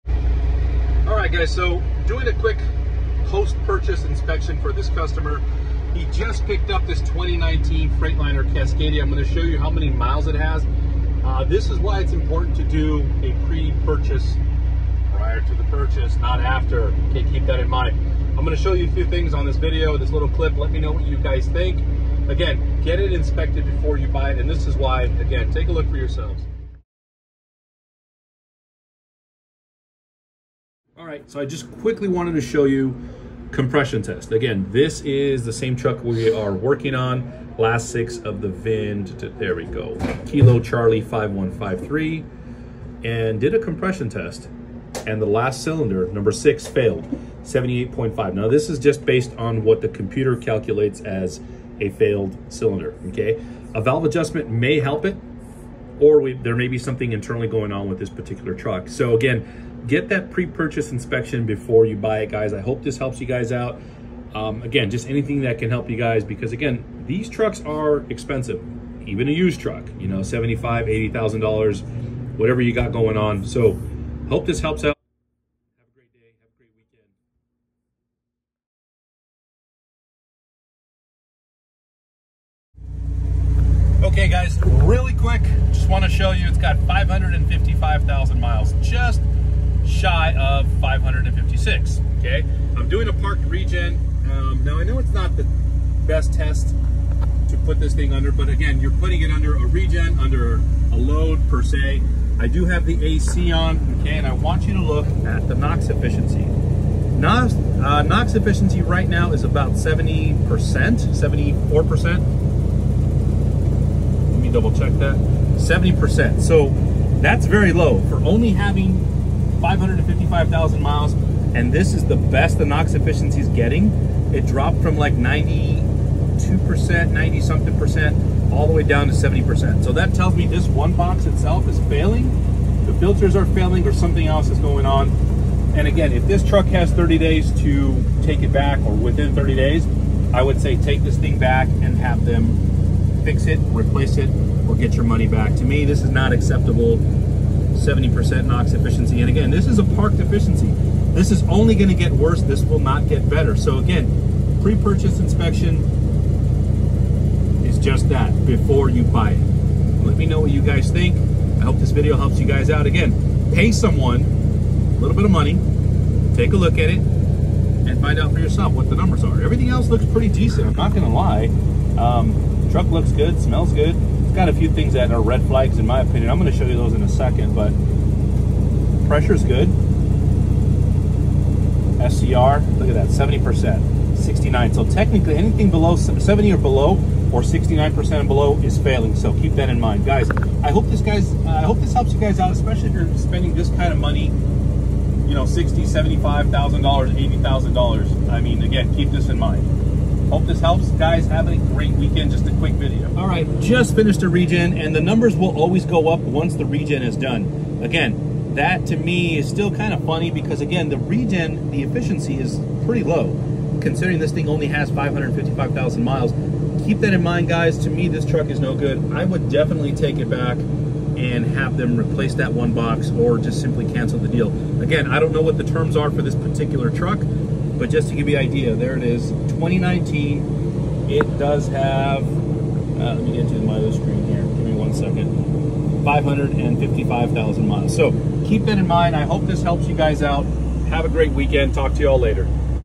Don’t make this mistake when buying a new to you truck!
Customer brought me a Cascadia to look at after he purchased it. Lucky for him he has w0 days to look at it and hopefully get it fixed by the dealership it was purchased from.

I performed a compression test which show cylinder 6 failed with 78%

NOx efficiency started strong at 92% then dropped as low as 68%. This could be a bad NOx sensor, failed DPF , failed One Box.  Don’t be afraid of turning on the AC to help strain the engine.

This is why a pre purchase inspection to catch as many issues as possible.